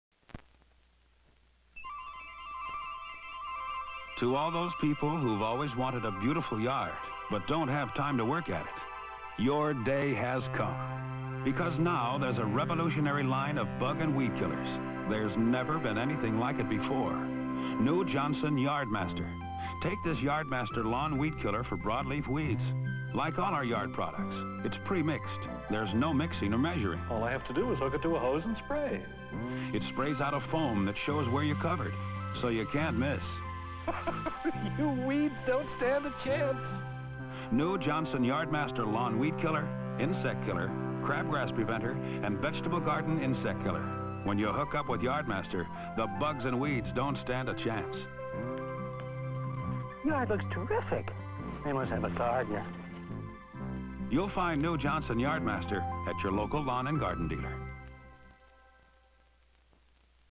VINTAGE DISCONTINUED JOHNSON WAX PRODUCT - YARD MASTER WEED KILLER COMMERCIAL
What a great product. I always hated using sprays on the lawn, mainly because I couldn't tell if I overlapped an area, or if I missed an area. This product sprays the weed killer on the lawn, and it sticks to the grass as a white-colored foam. It is a perfect product for a MORON homeowner, just like myself. It is too bad that it was never released nationally. To the best of my knowledge, it was never available in my area. Maybe the company could re-introduce it. There are a lot of MORON homeowners out there, who have a big need for a foolproof (MORON-proof) weed killer.

I get a kick out of the milk delivery truck, which is seen just prior to the homeowner opening his garage door. Believe it or not, in my area there are TWO companies that deliver milk to homeowners. At least one of them is making a lot of money for the owner. He has reportedly spent a couple of hundred million dollars trying to win a political office. He has run for Governor, U.S. Senator & U.S. Congressman. After losing 5 elections (he was nicknamed THE MILK DUD), he finally won an election. He is one of our new STATE Senators. He couldn't win a big election, but he did win the BOOBY PRIZE.